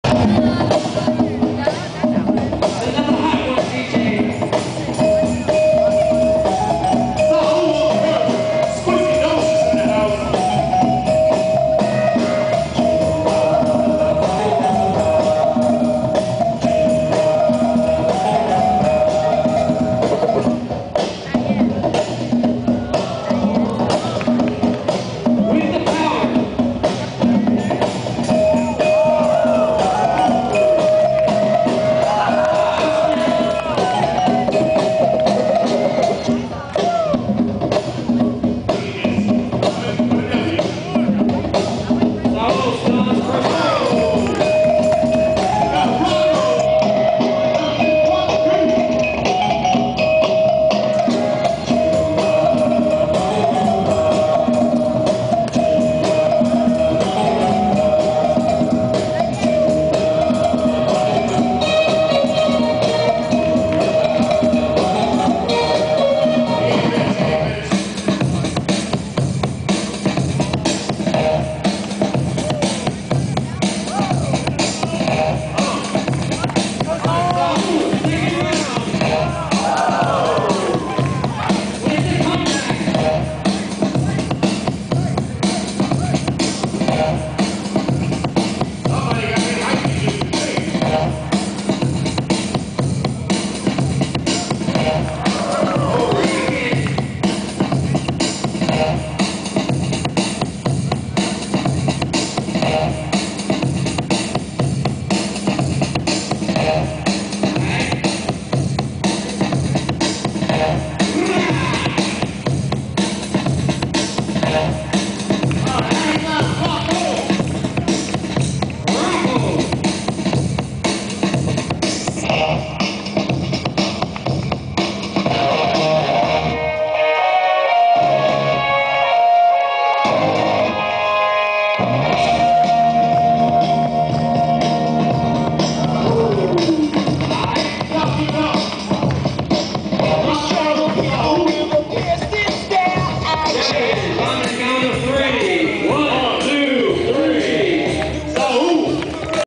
Piedmont Drop III - Saewl (SD) vs. Fernando (OP)
Saewl takes it.